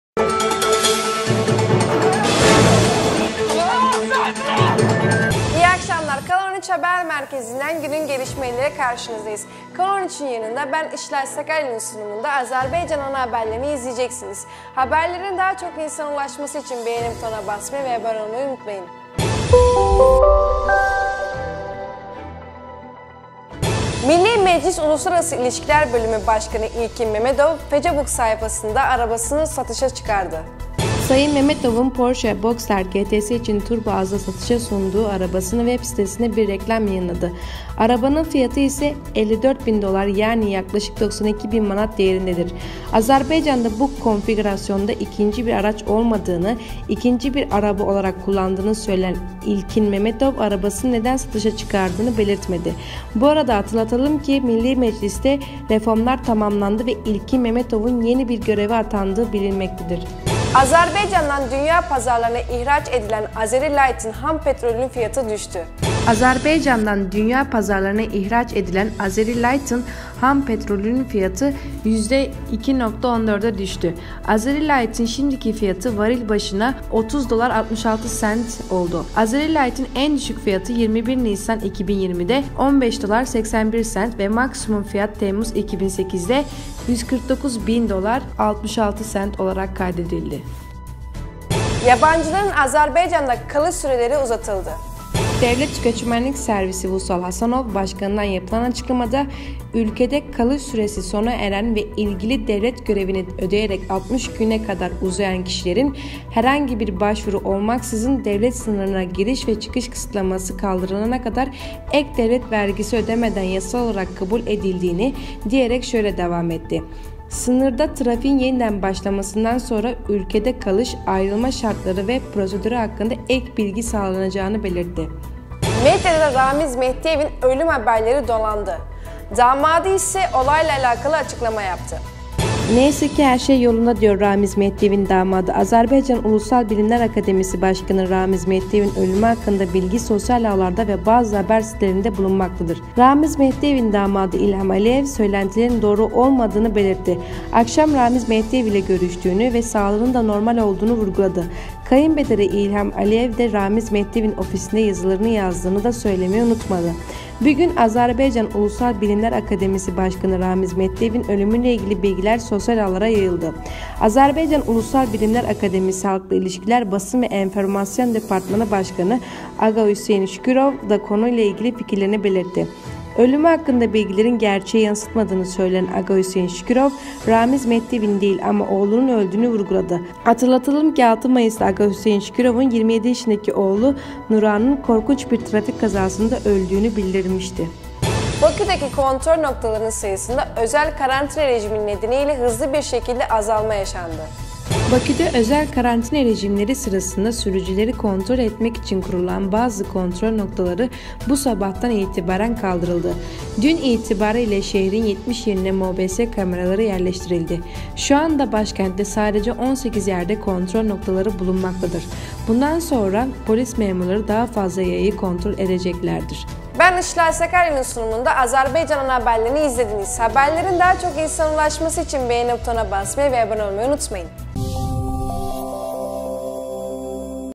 Ramiz Mehdiyevin ölüm XƏBƏRİ GERÇƏKDİRMİ?Milli Məclisin şöbə müdiri bahalı maşınını FACEBOOKda satır
1- Milli Meclis daire başkanı pahalı arabasını satışa çıkardı
2- Azerbaycan petrolü yeniden ucuzladı
3- Yabancıların Azerbaycan'da kalış süreleri uzatıldı.
4- Medyada RAMIZ MEHDIYEV'İN ÖLÜM HABERLERİ dolandı. damadı ise Açıklama yaptı.
5- Bakü'deki kontrol noktaları karantina rejimi nedeniyle azaltıldı